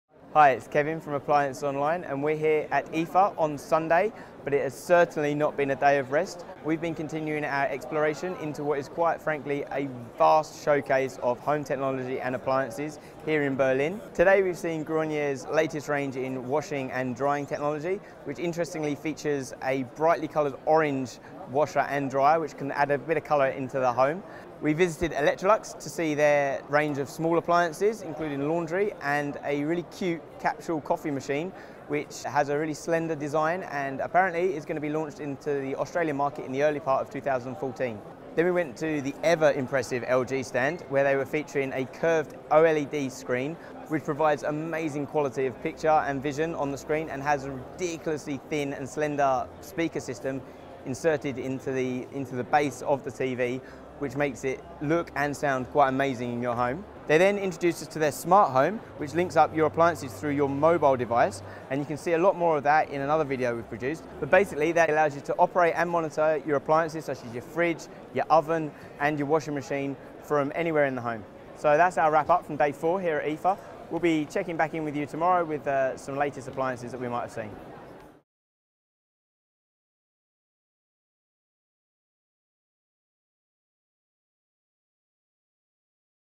LG's curved OLED TV and Smart Home, plus Electrolux's coffee machine, @IFA 2013 - Appliances Online
It's day four here at the IFA trade show in Berlin and we at Appliances Online have seen another batch of the best the appliance industry has to offer.

In our sights today was the smalls range from Electrolux, which features a cute capsule coffee machine, the latest Gorenje washer dryers with the orange design, and LG's curved OLED TV.

LG also showed us their smart home appliances which can be operated and monitored through your smart phone.